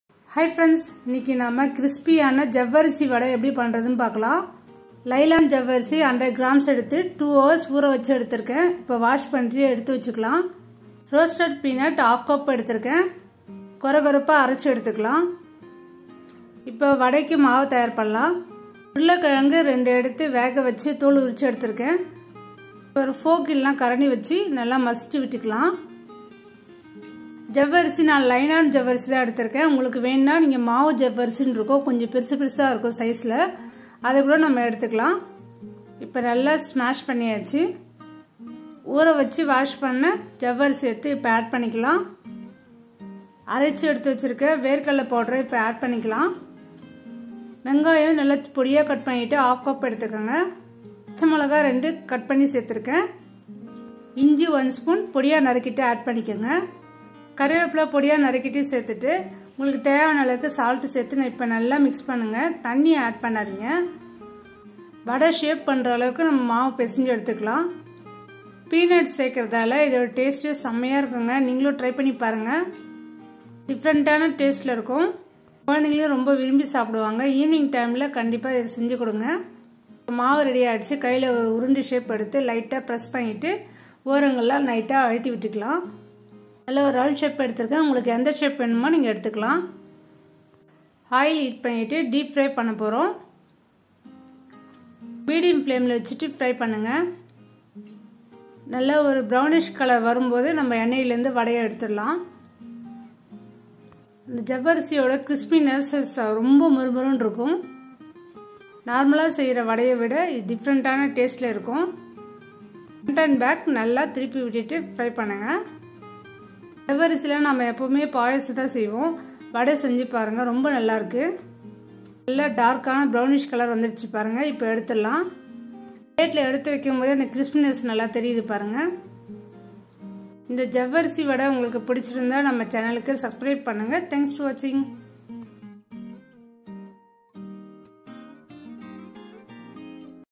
Crispy Sabudana Vadai/Javvarisi Vadai/Javvarisi Snaks
Ingredients
javvarisi - 100grams
roasted peanut - 1/2cup
Boiled potatoes - 2
chopped onions - 1cup
chopped ginger - 1spoon
curry leaves
salt - 1/4spoon
chopped green chillies - 1spoon